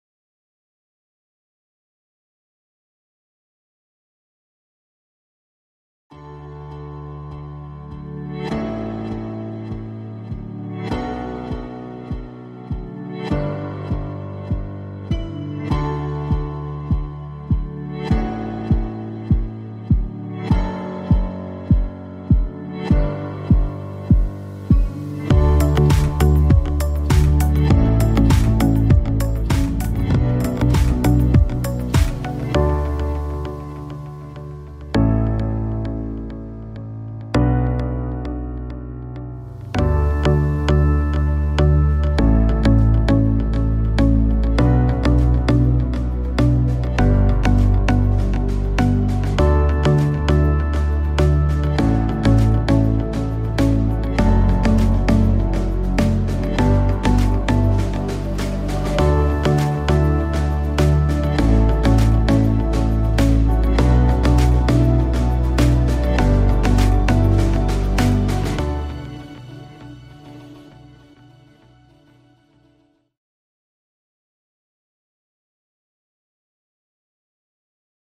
Déambulation automnale
Partir à la découverte des paysages altiligériens en automne : des lacs aux montagnes.